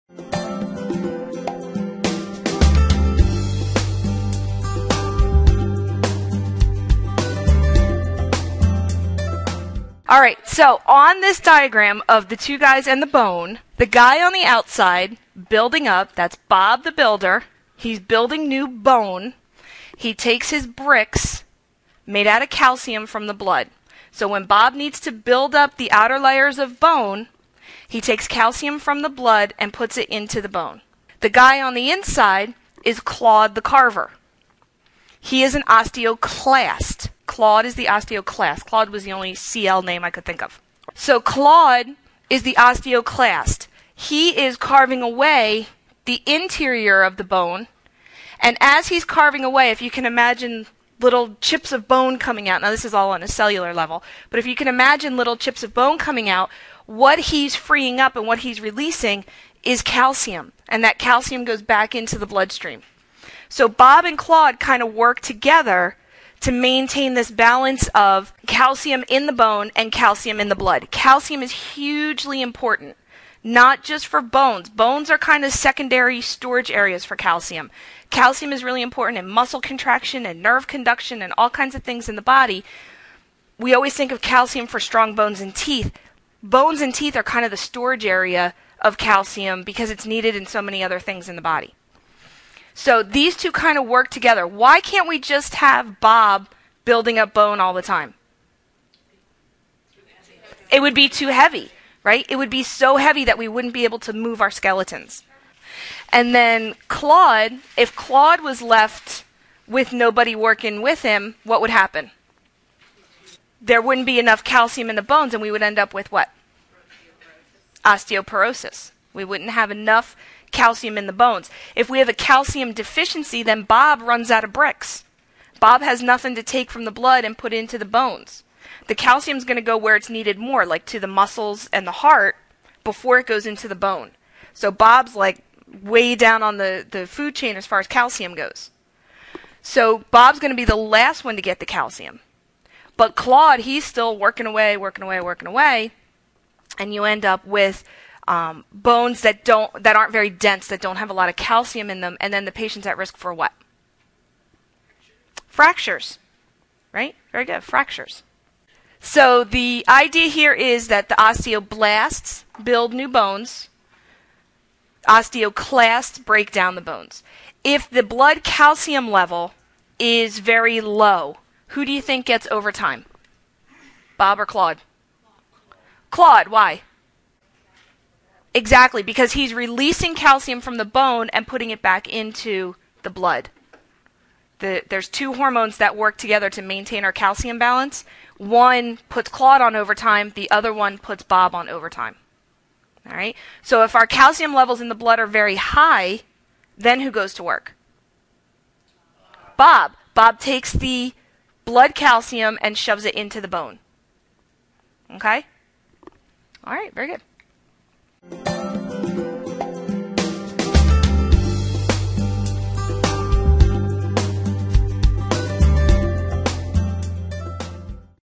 Bone remodeling (Bob and Claude)
A (very) basic overview of bone remodeling and resorption using the story of "Bob and Claude". The image is from "The Human Body in Health and Illness 4th ed" (2010) from Elsevier